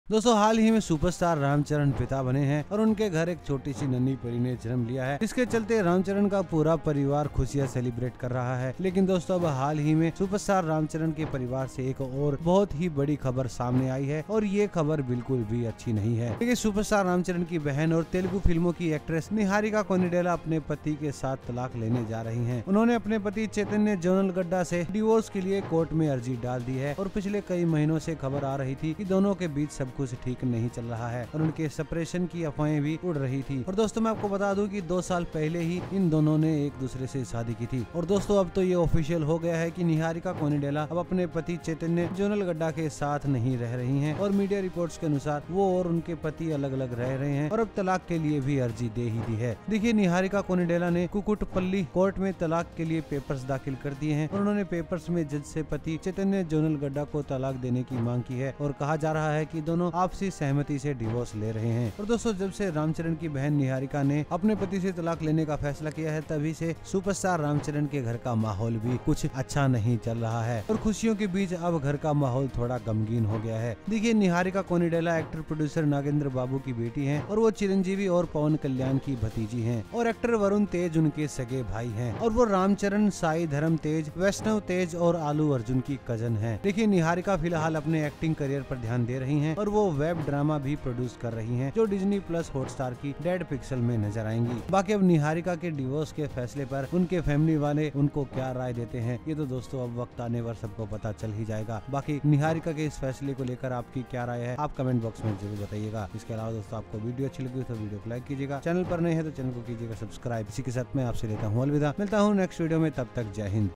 रामचरण की बहन पर टुटा दुखों का पहाड़ | Ram Charan’s sister Niharika Konidela Announces Divorce
TIMESTAMPS:
रामचरण की बहन पर टुटा दुखों का पहाड़ 00:00​
Ram Charan’s sister Niharika Konidela Announces Divorce 1:23